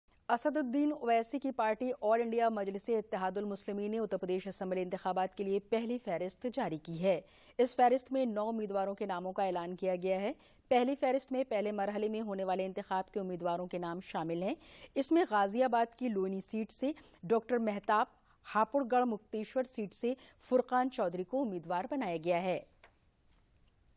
असदुद्दीन ओवैसी की पार्टी ने उत्तर प्रदेश विधानसभा चुनाव के लिए पहली सूची जारी की
असदुद्दीन ओवैसी की पार्टी ऑल इंडिया मजलिस-ए-इत्तेहादुल मुस्लिमीन ने उत्तर प्रदेश विधानसभा चुनाव के लिए पहली सूची जारी की है। पहली सूची में नौ उम्मीदवारों के नामों की घोषणा की गयी है।